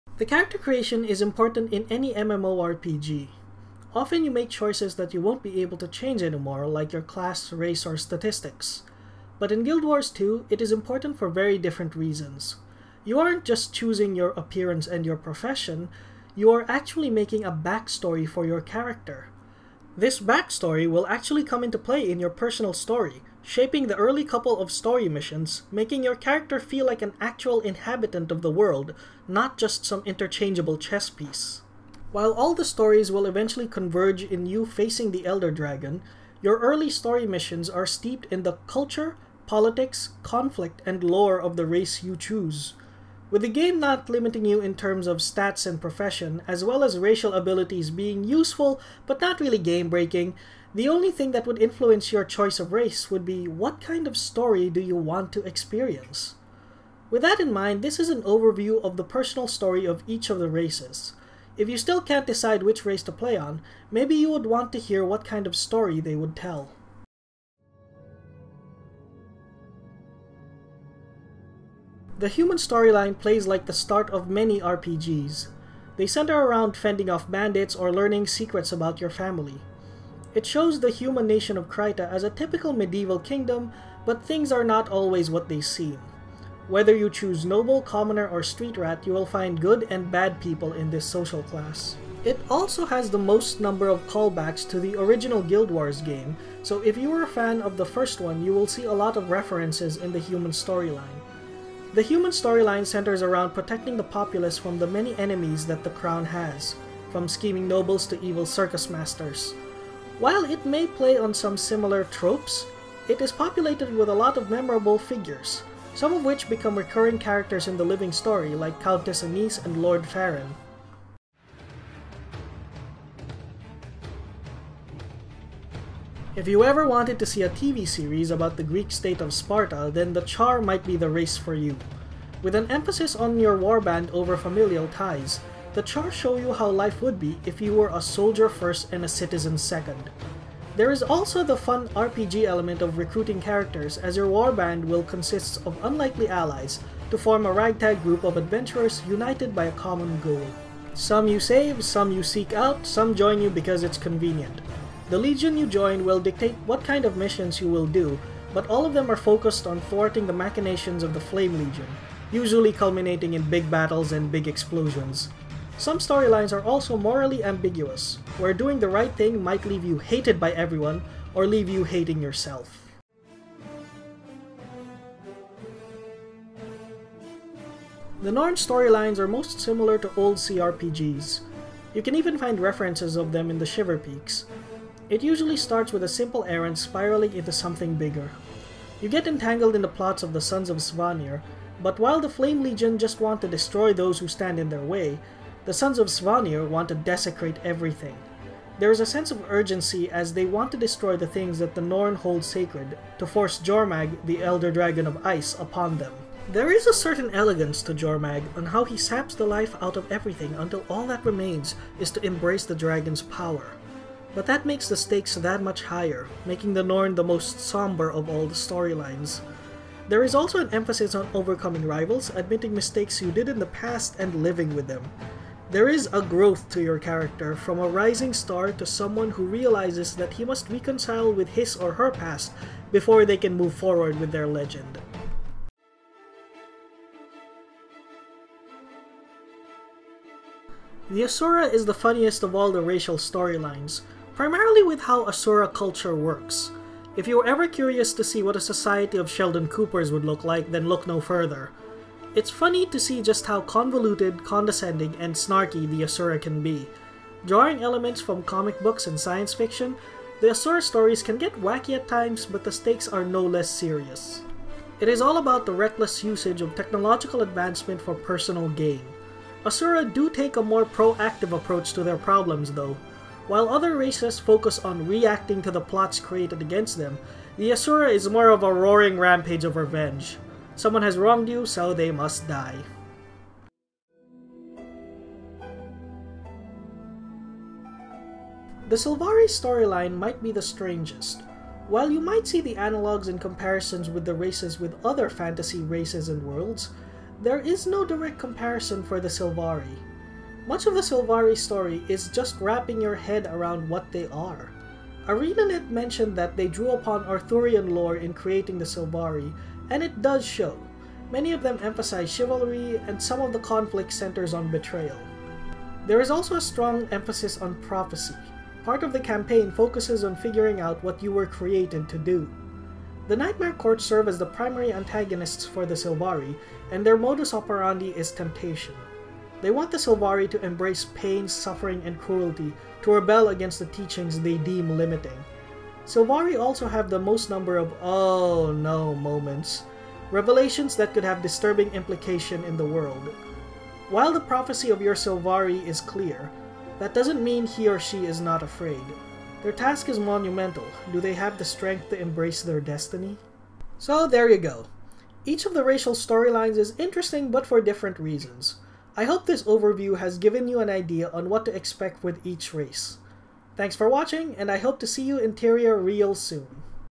Guild Wars 2 Racial Storylines - what to expect
If you are curious about the storylines of each of the races in Guild Wars 2, this is a overview on the themes, plots and highlights from each of them.